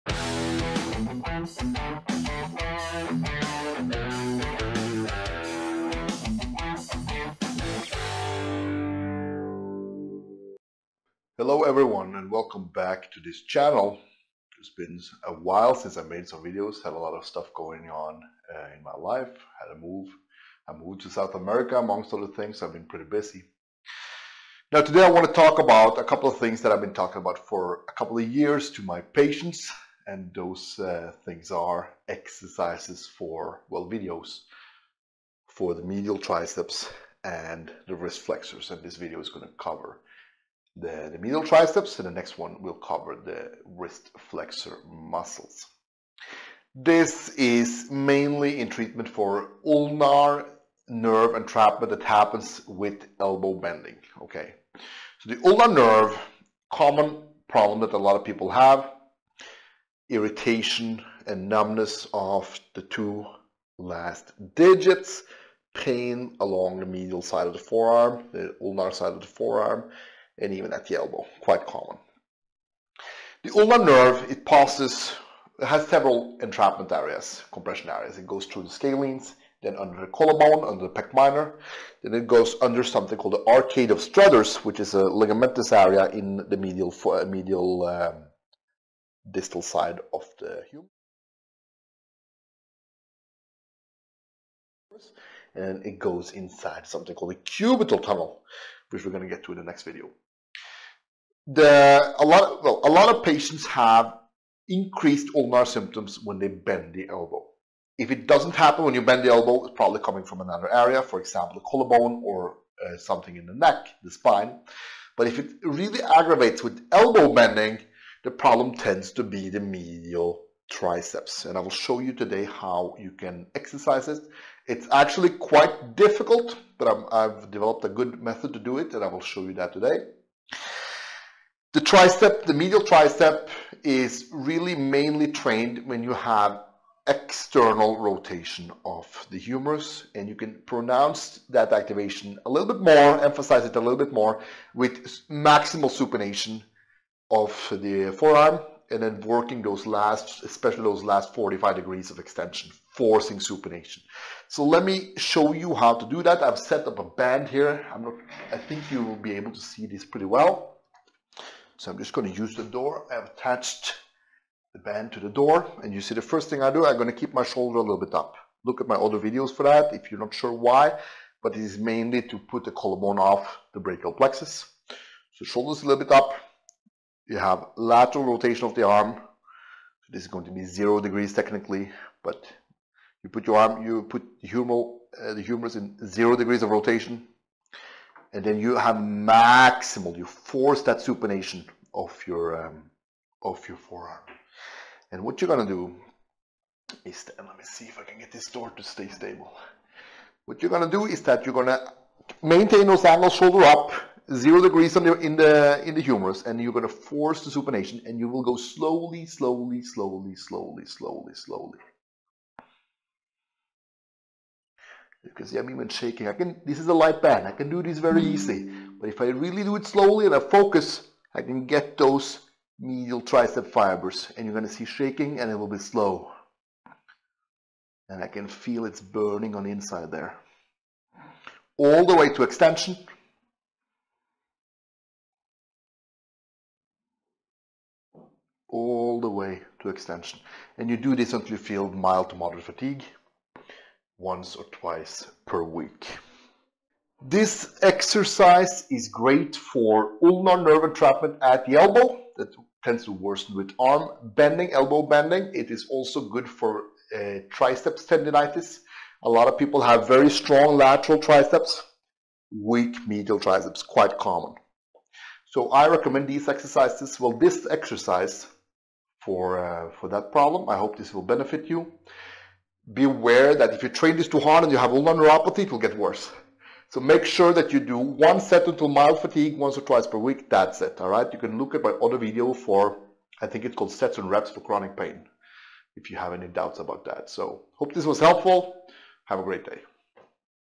Medial triceps rehabilitation for triceps tendinitis and ulnar nerve entrapment & dislocations
Weakness of the medial triceps is very common. The medial triceps is like the VMO of the thigh; it activates the best during humeral external rotation and at the last degrees of extension.

Its weakness is commonly associated with triceps tendinopathy, ulnar nerve entrapment as it courses through the arcade of Struthers, as well as medial dislocations of the nerve or even the medial triceps itself (severe circumstances).